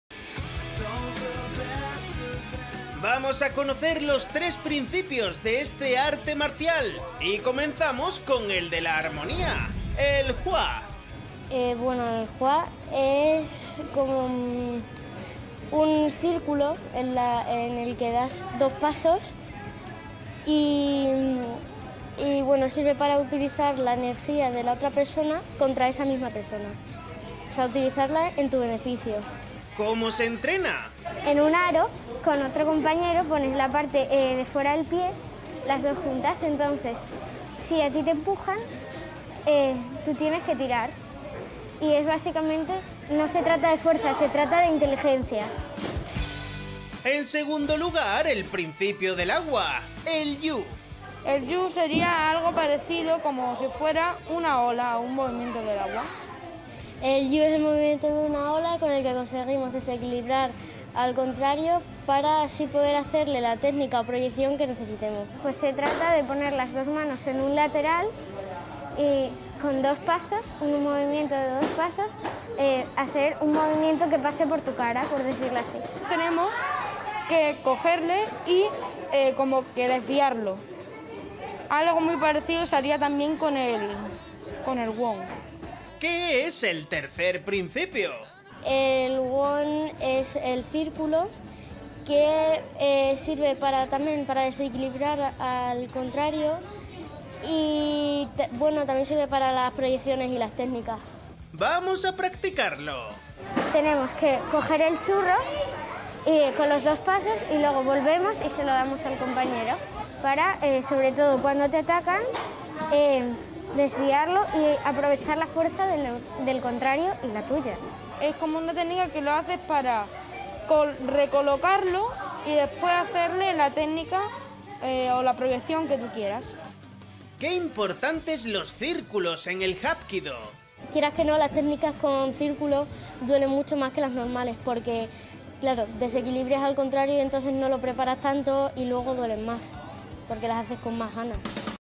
Aprendemos el arte marcial tradicional Hapkido | Generación Deporte
Conocemos los tres principios del Hapkido, un arte marcial tracidicional. De la mano de su maestro Carlos Leal, los chicos y chicas nos enseñan algunas técnicas y consejos.

¿Te apuntas al Hapkido?